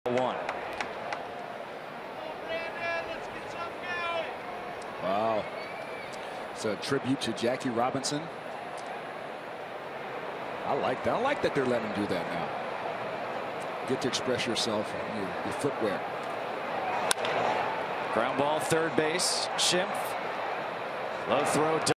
SD@ATL: Phillips sports Jackie Robinson-themed spikes
Brandon Phillips displays his Jackie Robinson-themed spikes as a tribute on Jackie Robinson Day